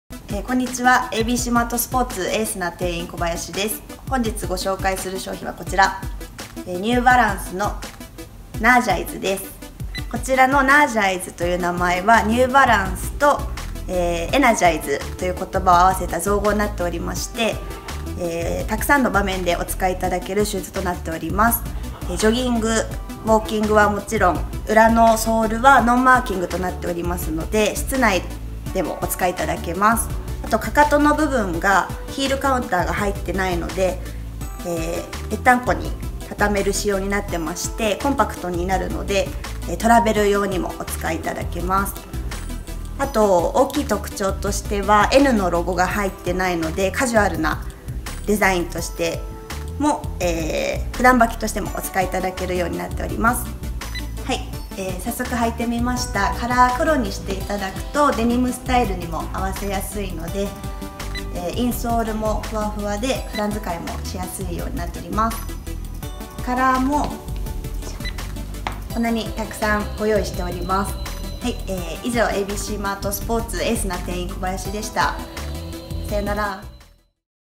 【NEW BALANCE】スポーツに、ファッションに、旅行に!!汎用性抜群の最新作シューズ!!【NERGIZE/ニューバランス】
業界最速!? スニーカーパイセンレビュー

NEW BALANCE
NERGIZE

カラー：
THISTLE GY(AG)
*BLACK/WH(BK)着用
FIJI (FH)手持ち
*LIGHT GRAY(OH)
*NAVY(PH)
SOLAR YE(YH)

（￥7,900+tax.）

是非店頭でお試しください！！

ABC-MART SPORTS